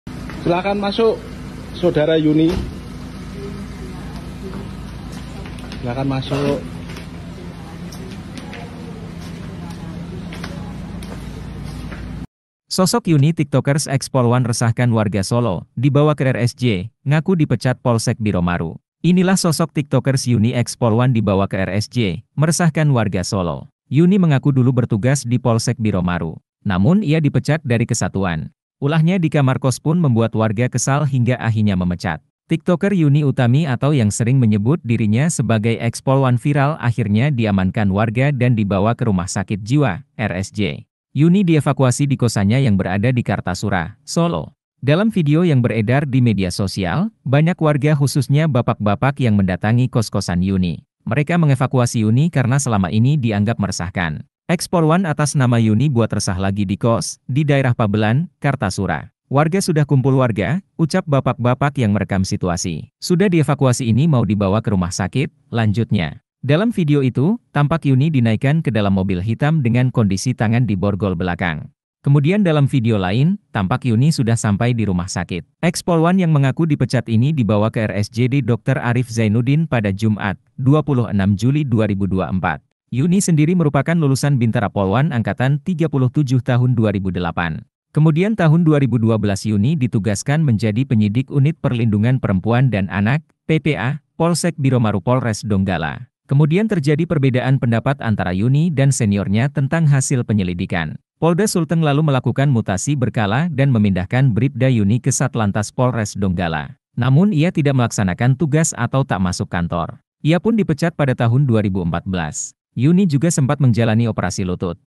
Sosok Yuni Tiktokers Eks Polwan Resahkan Warga Solo, Dibawa ke RSJ, Ngaku Dipecat Polsek Biromaru
Inilah sosok Tiktokers Yuni Eks Polwan dibawa ke RSJ, meresahkan warga Solo.
Yuni mengaku dulu bertugas di Polsek Biromaru.
Namun ia dipecat dari kesatuan.

Program: -
Host: -
Editor Video: Muhammad Maulana Ahmad Al H
Uploader: -